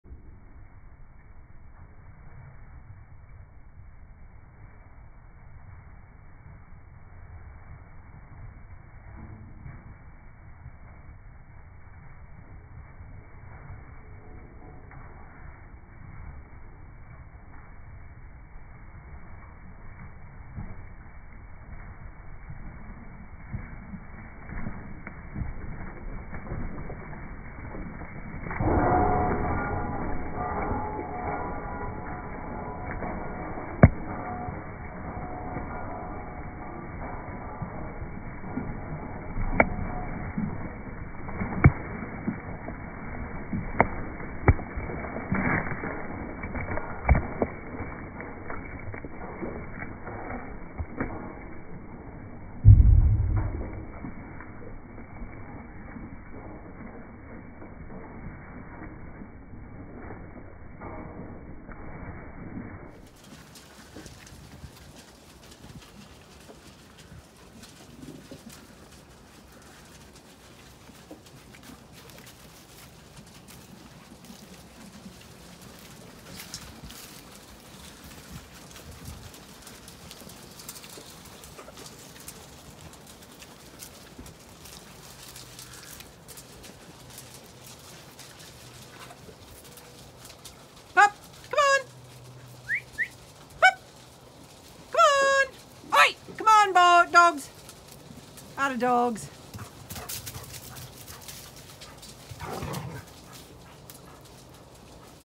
Stampeeeeede for ewes dinner this evening
They’re soon to be housed as lambing time approaches.